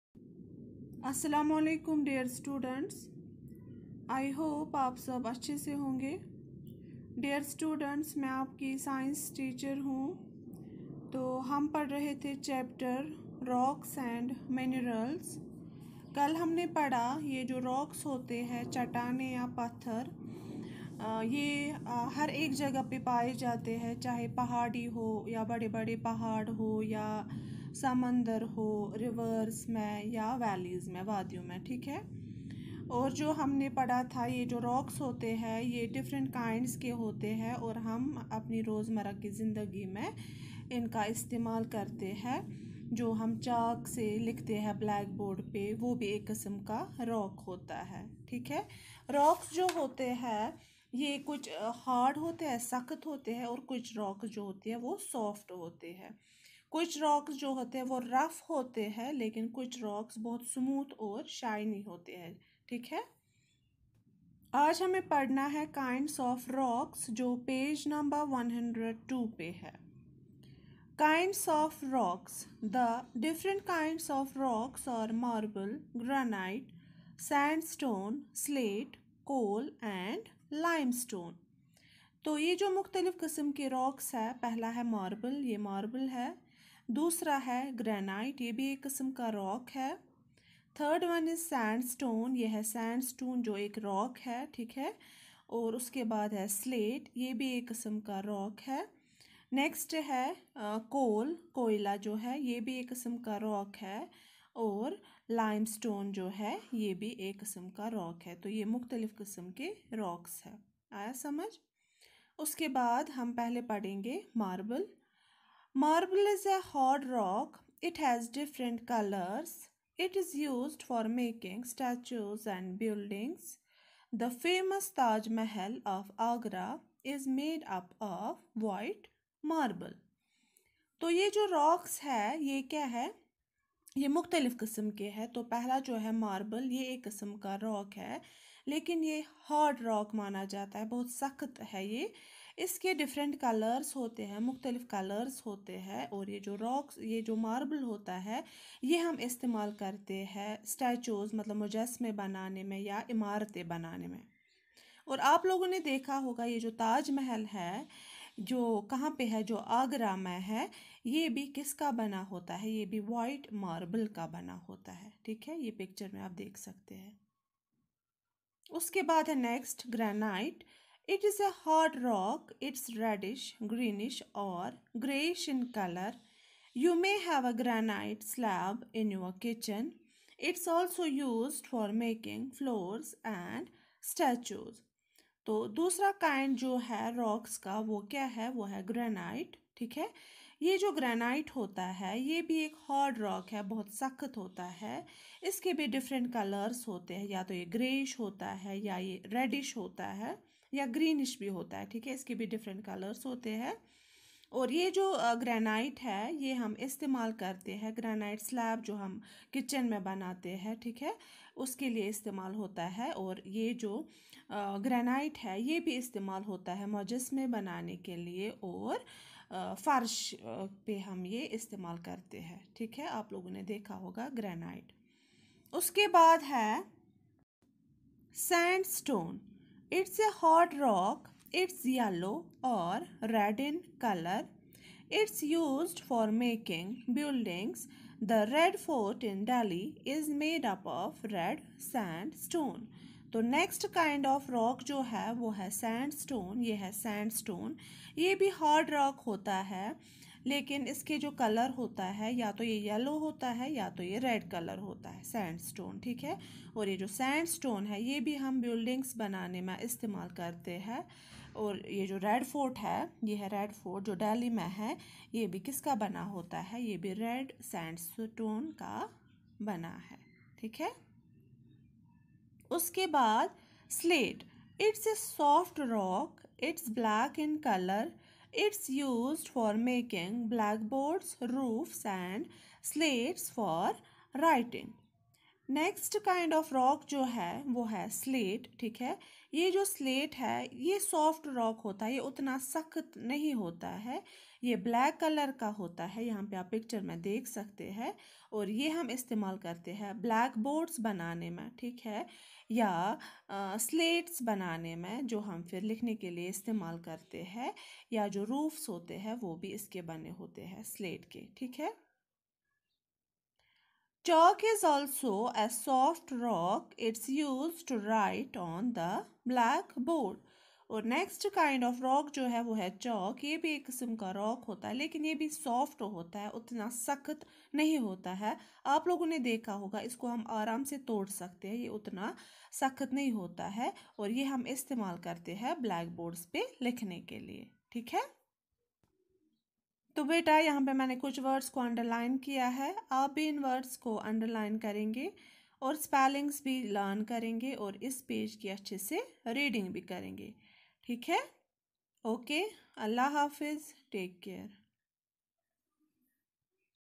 10_FEI_2nd_T2_CH14_VL2_Rocks and Minerals
Lecture on the topic Rocks and minerals by Ruqaiya Gani, teacher FEI